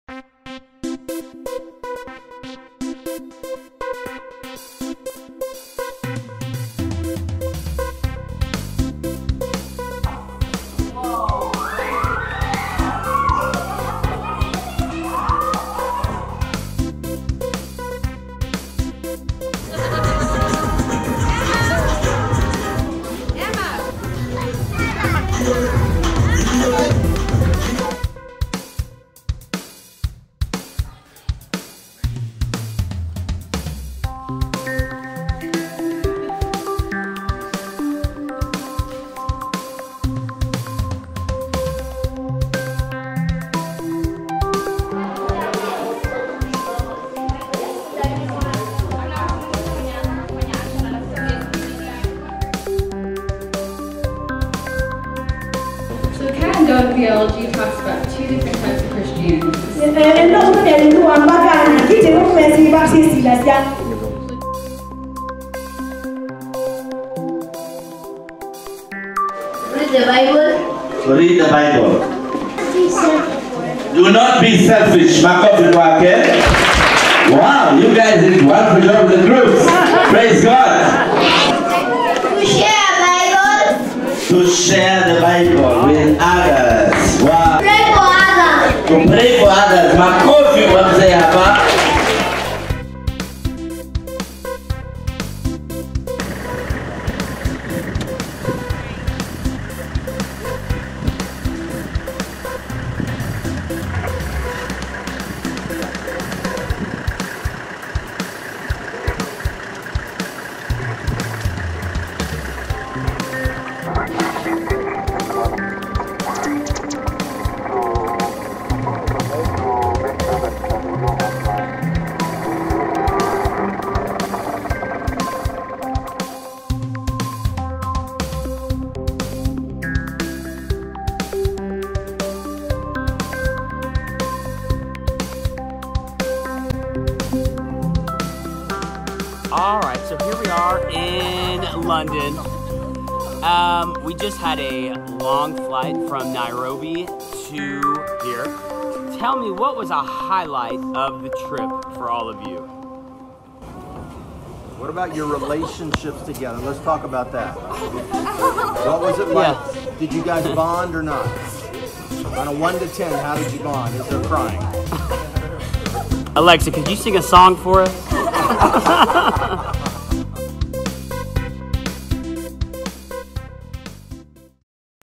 2019 Kenya Advertising Video
Homeschoolers, come to Nairobi, Kenya with us to teach Cat and Dog Theology to precious children there!